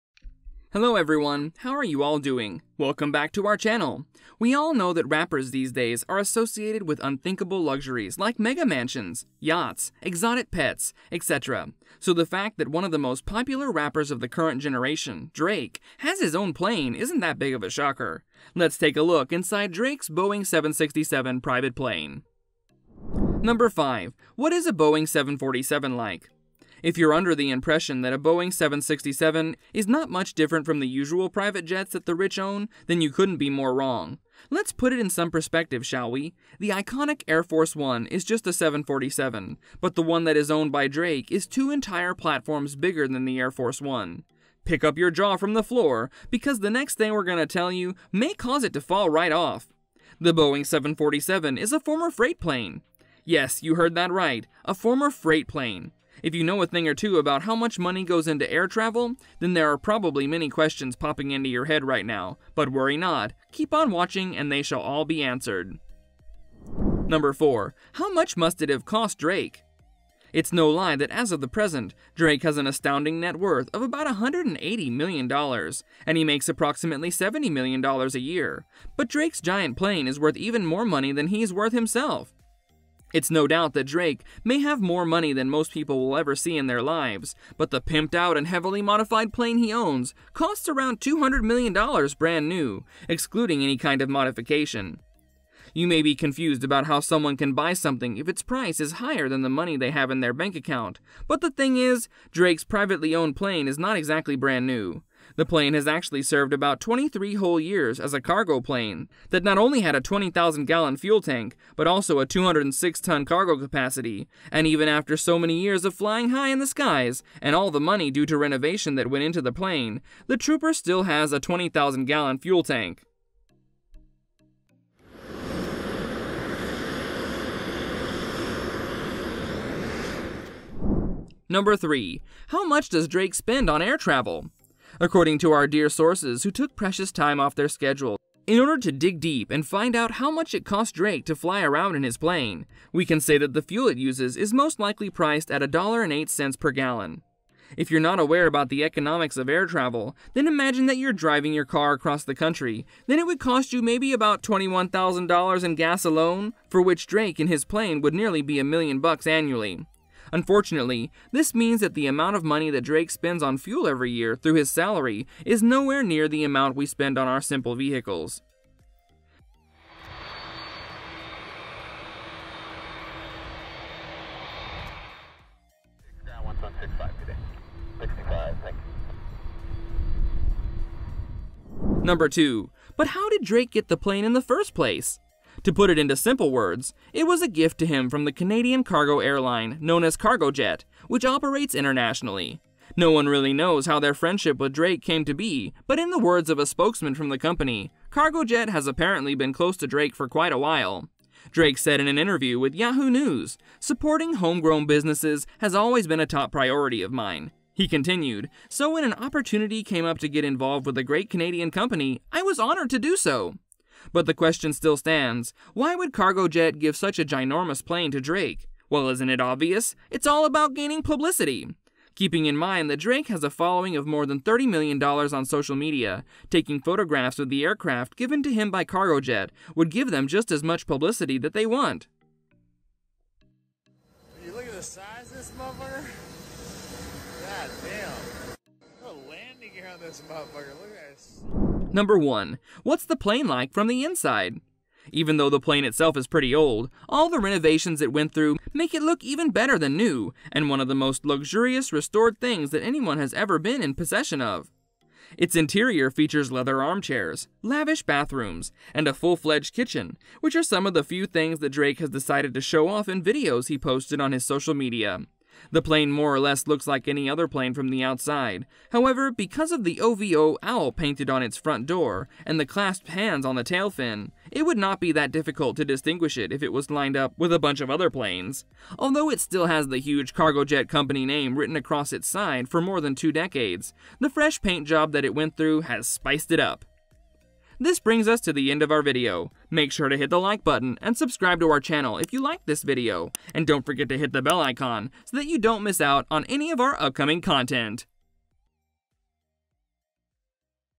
Inside Drake's Boeing 767 Private Plane!
HI there, and welcome to Nothing Finer1 Drake has become one of the greatest rappers of his generation. And with a net worth of $250 million, he's been able to afford a life of luxury. So in today's video, we will be taking a closer look inside his Boeing 767 private plane.